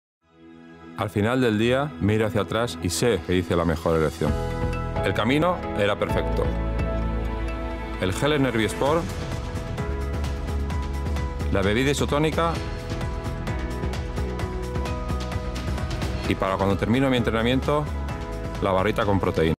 Diseñado para el deporte
Spot tv Enervit Spagna 2018
Testimonial Miguel Indurain
Regia Francesco Mansutti
Fotografia Piergiorgio grande
Una produzione FilmArt Studio